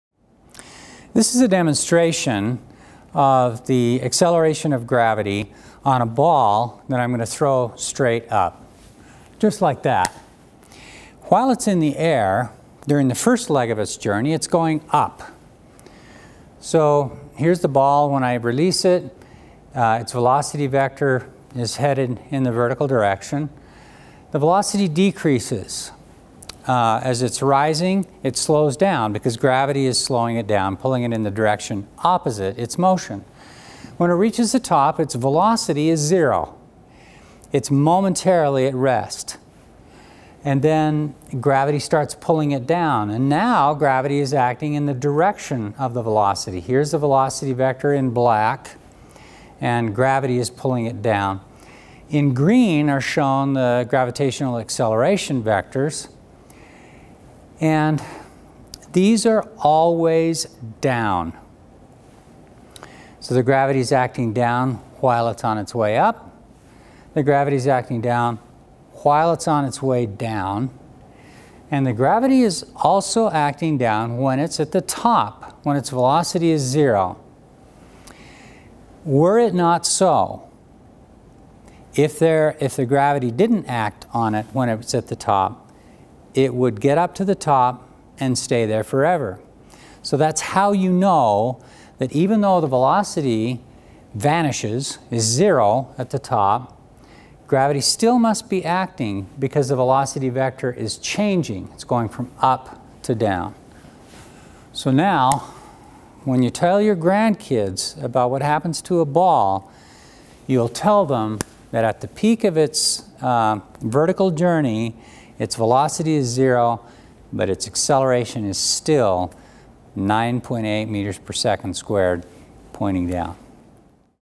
Kinematics Demo: Tennis Ball Projected Upward
This is a demonstration of the motion of a tennis ball that is thrown upward, together with a discussion of the velocity and gravitational acceleration vectors at various points in its trajectory.

It was created at Utah State University by Professor Boyd F. Edwards, assisted by James Coburn (demonstration specialist), David Evans (videography), and Rebecca Whitney (closed captions), with support from Jan Sojka, Physics Department Head, and Robert Wagner, Executive Vice Provost and Dean of Academic and Instructional Services.